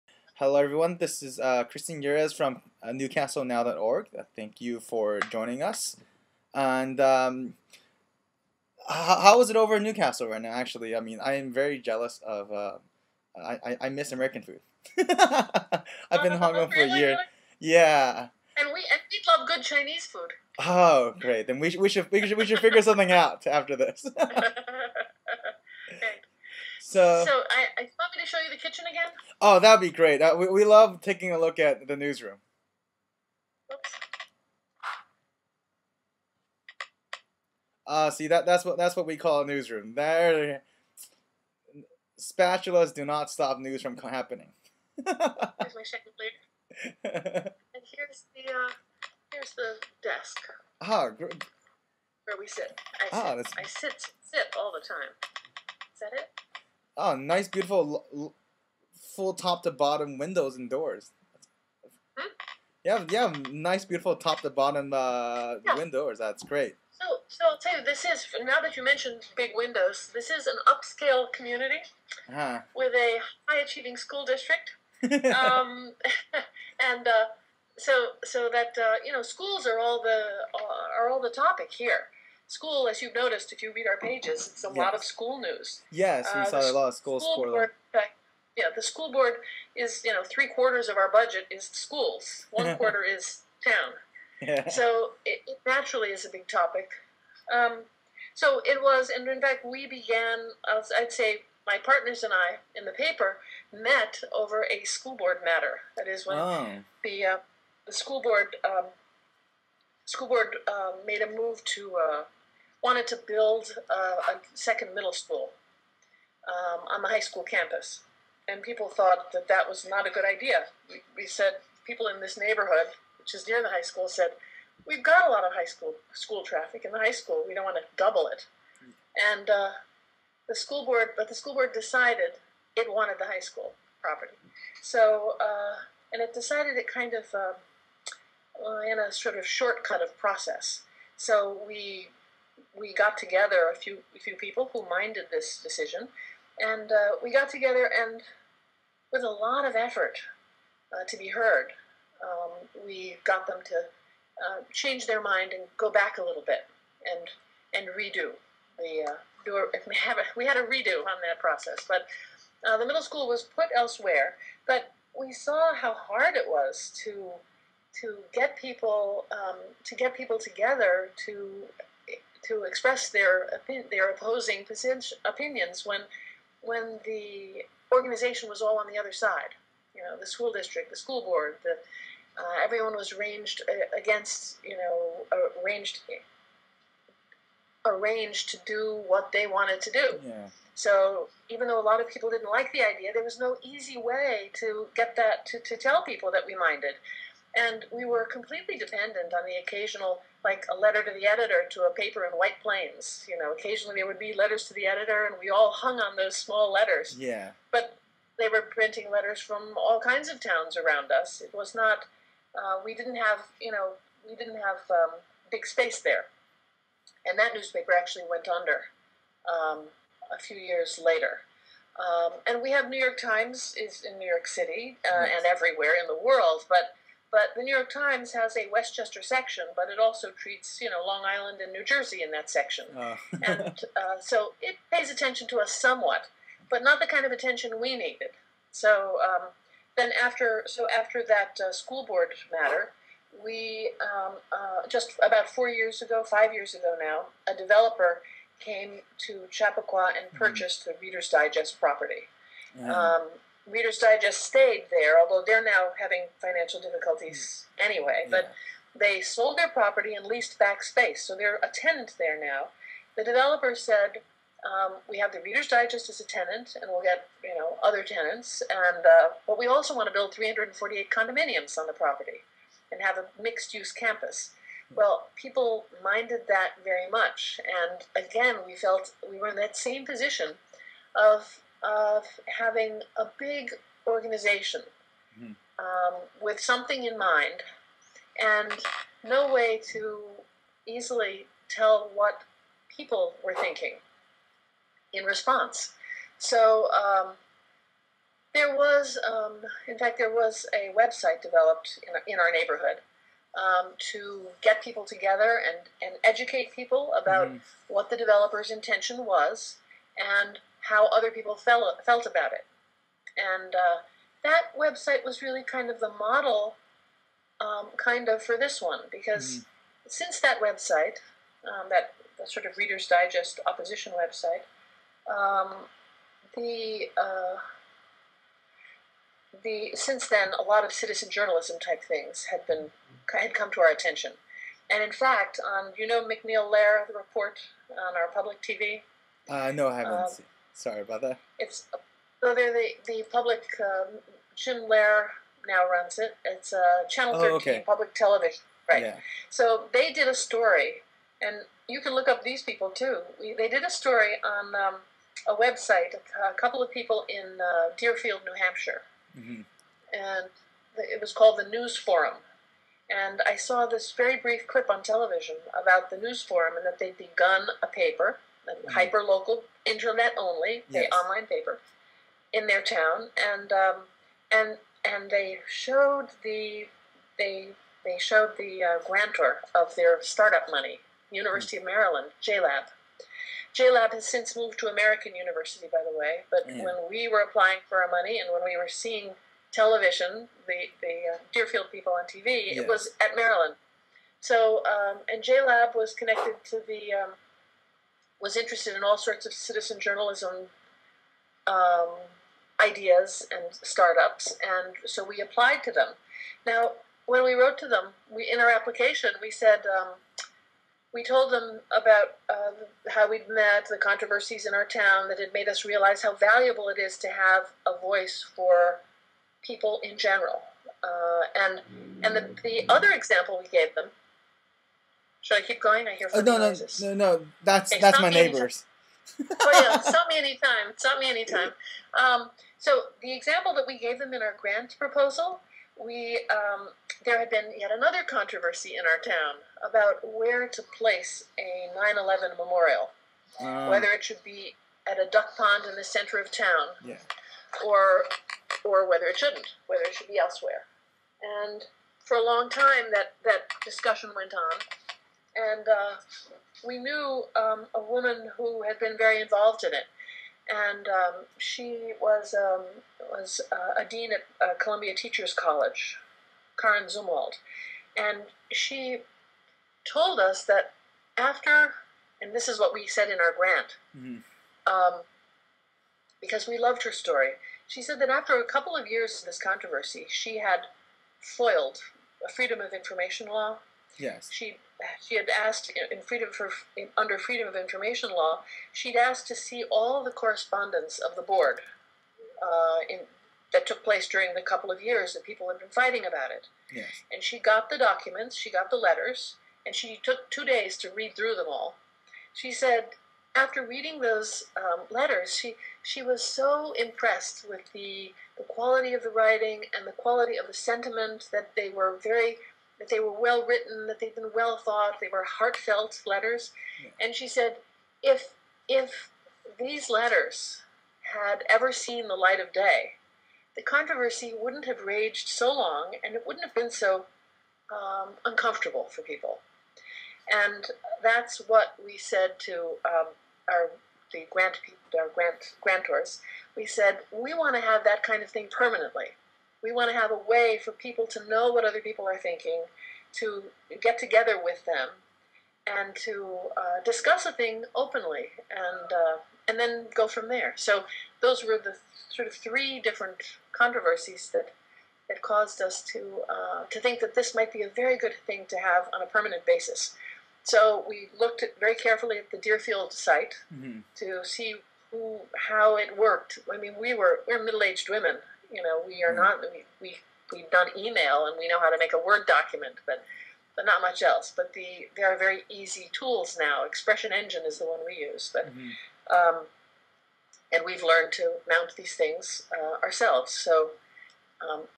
Newcastle Now (Part 1)
A conversation with Christine Yeres, Managing Editor for Newcastlewnow.org